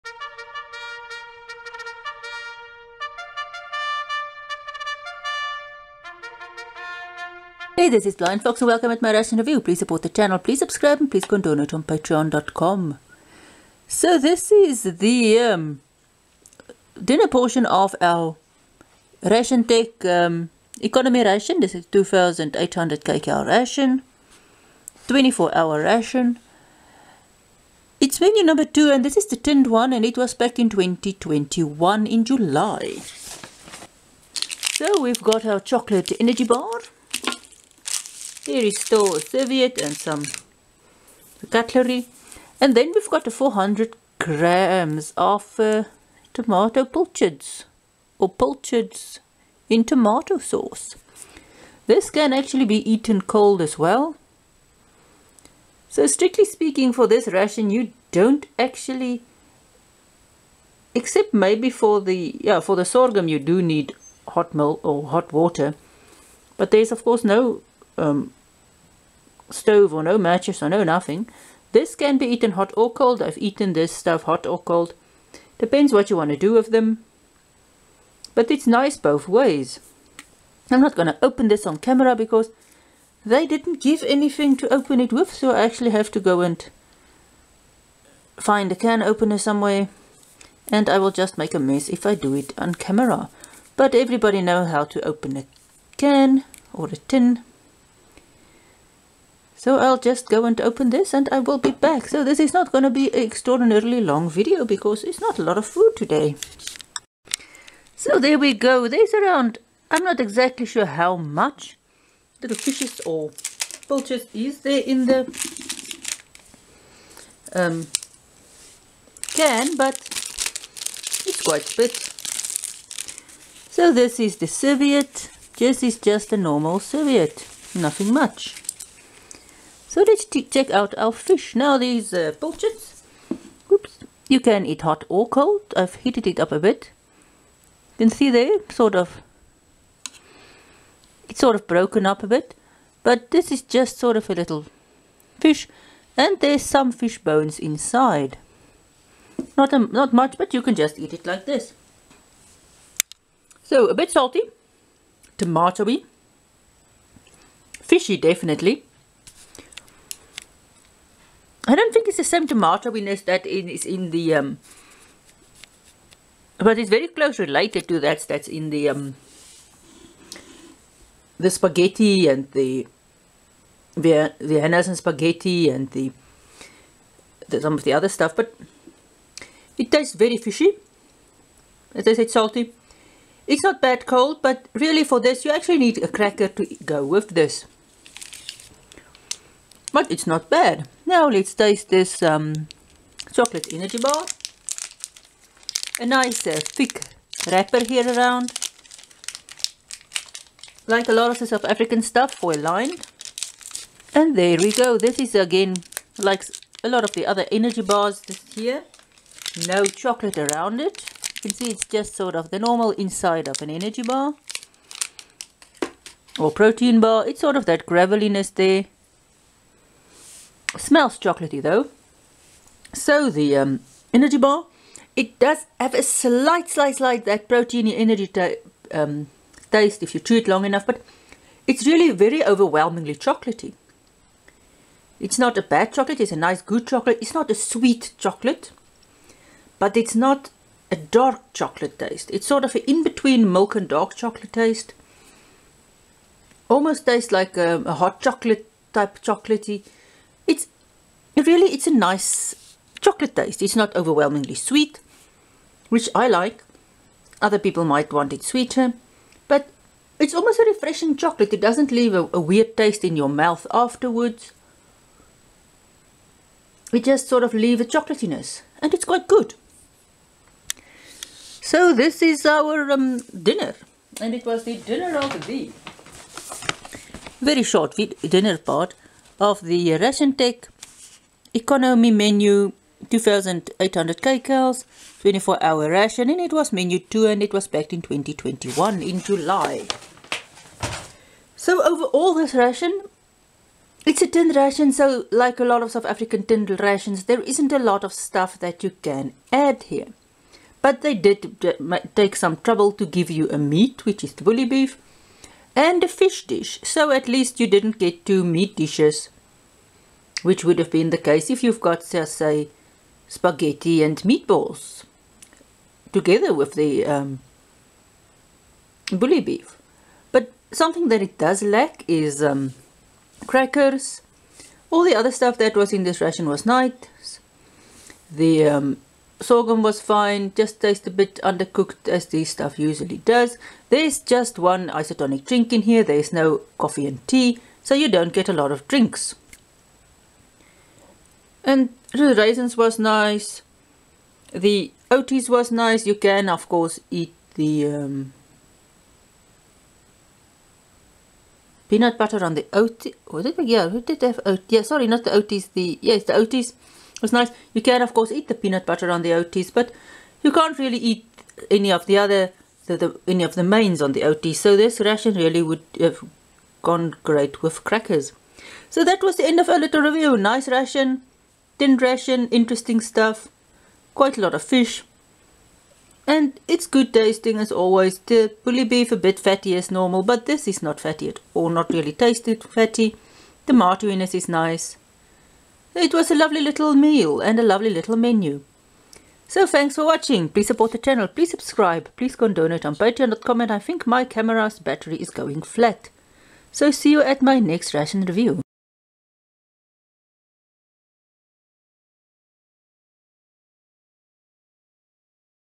South African Ration Review: 2021 RATIONTECH 24H Econo Menu 2: Dinner Part 4 of 4
First YouTube Review!!!
This is the new RationTech 2021 Ration Packs

Go buy some MREs on MRE Mountain and use my link or buy me a GIFT CARD:

Thank you to all of the ration reviewers who gave me this idea.